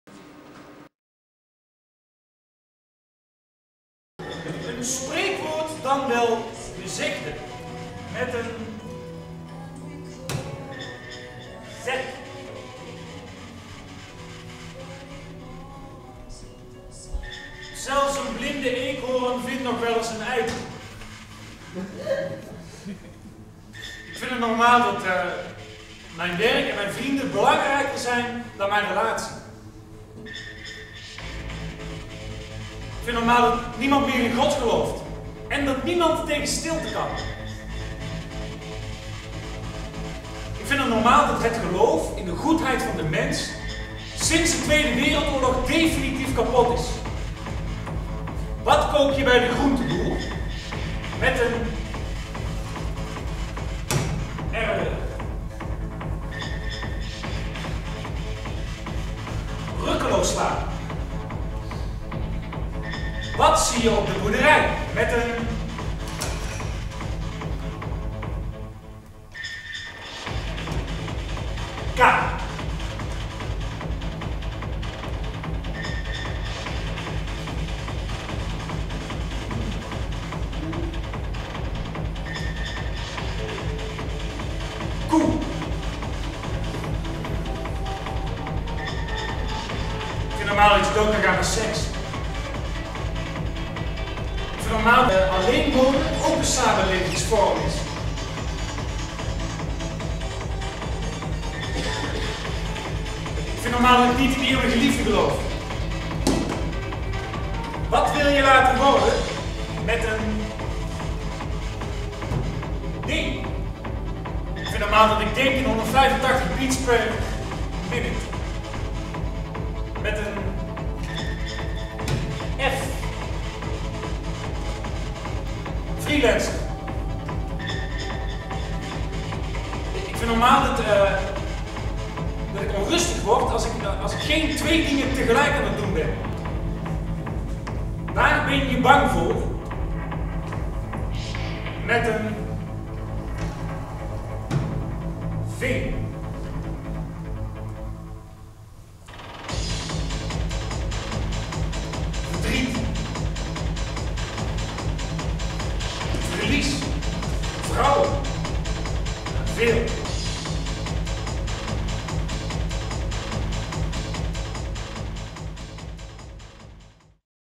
Over de liefde - theatervoorstelling Convoi Exceptionnel
Compilatie van de theatervoorstelling 'Over de Liefde' (2001) van Convoi Exceptionnel, waarin drie acteurs uit de nixgeneratie weigeren het failliet van de romantische liefde te accepteren. Concept en regie: Anke Boerstra. Vormgeving: Hetty van Bommel. Met: Hans Maas, Marcel Roelfsema, Ingrid Wender.